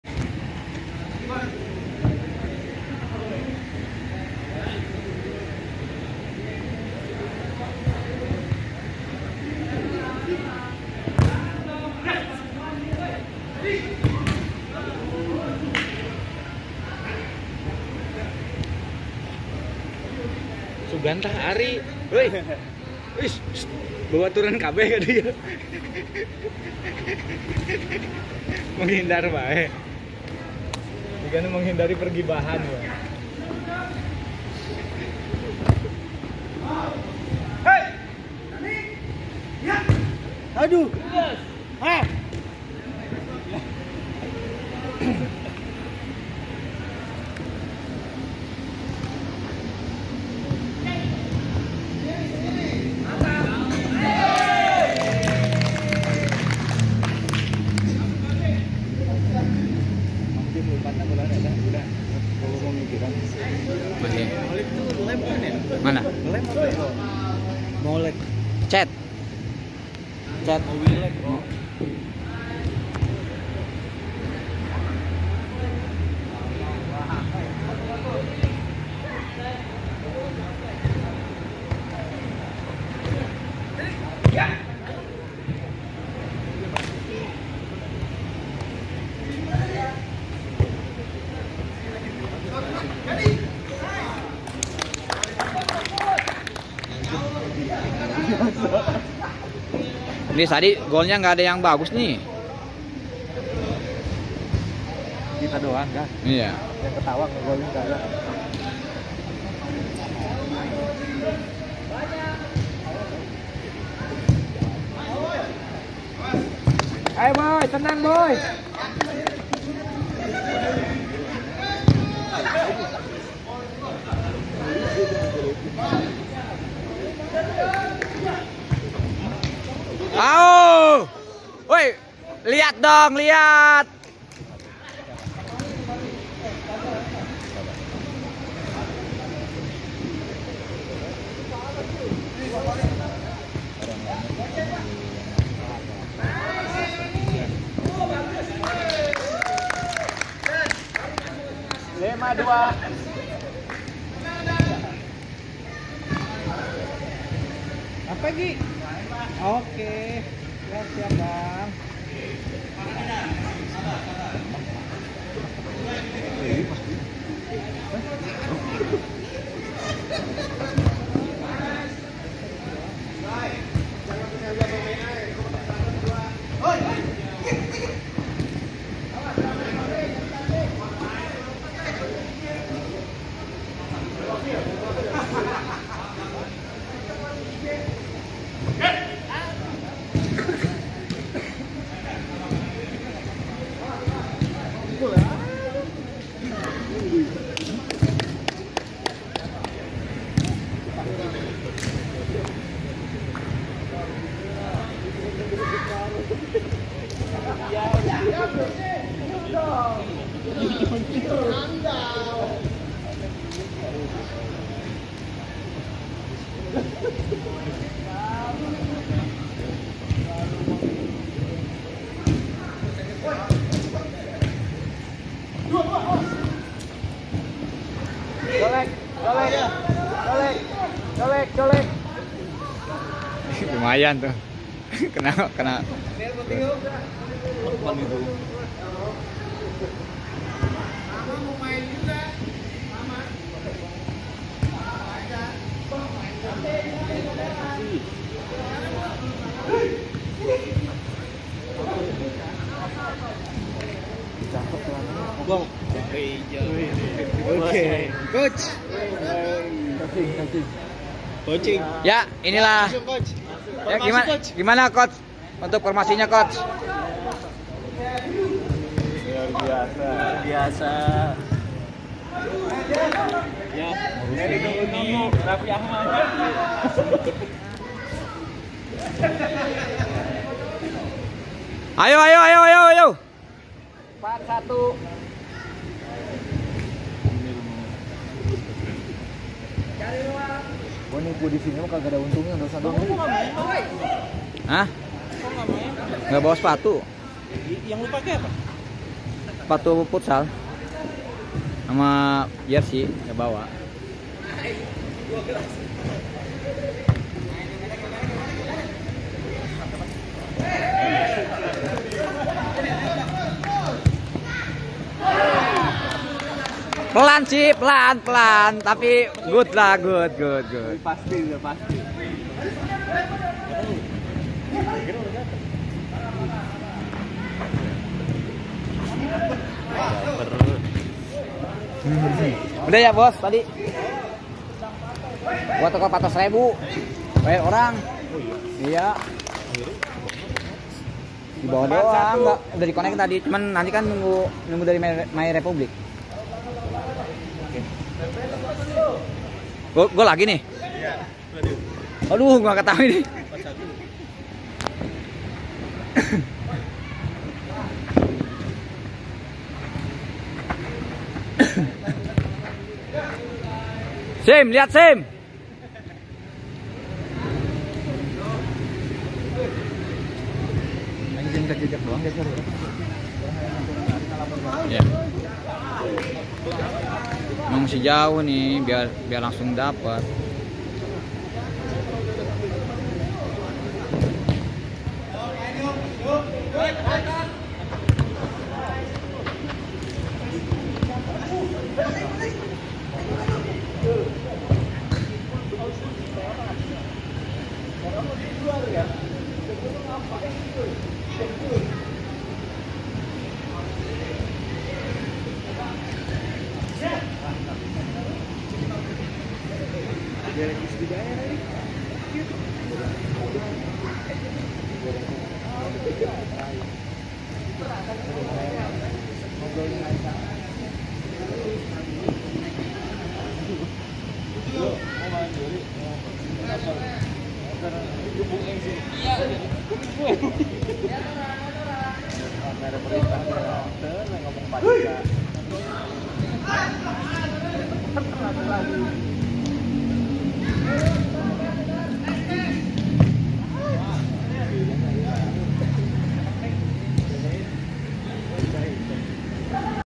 Pertandingan futsal penuh intrik⁉️Adu Goal & Adu Gengsi😤
Kegiatan setiap 1 minggu sekali untuk menjaga kondisi kebugaran dan kesehatan di team projek Housing landed